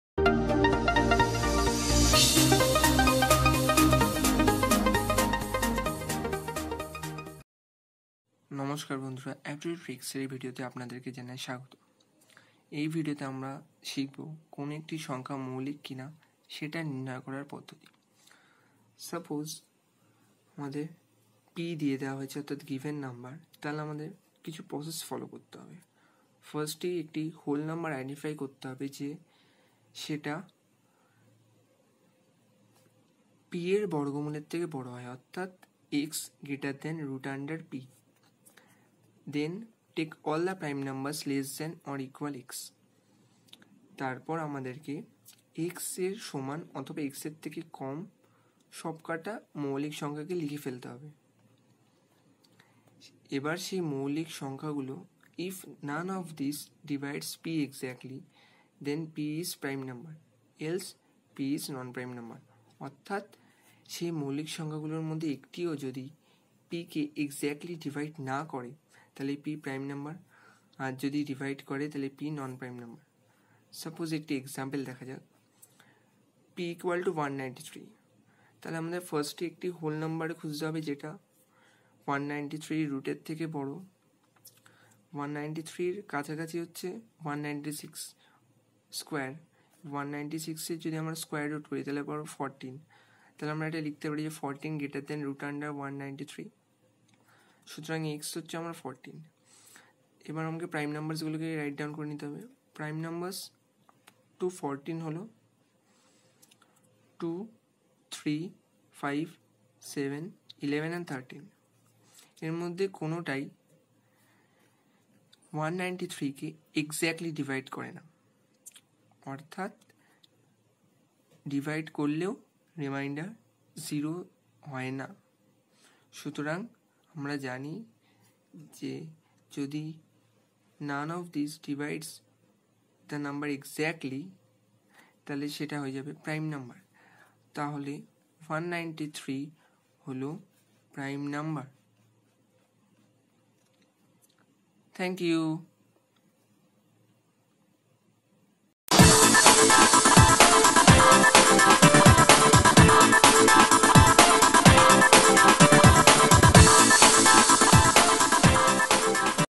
মৌলিক সংখ্যা বের করার নিয়ম| 2 মিনিটে | সহজতম উপায় | Bengali Tutorial | Aptitude Tricks
Hello! In this video titled PRIME NUMBER TRICKS, we will learn how to check Prime numbers. After watching this video, you will easily learn how to check prime numbers.
Hope you guys like the video, please give a thumbs up and do share with your friends... If you have any doubt feel free to comment, we will get back to you soon,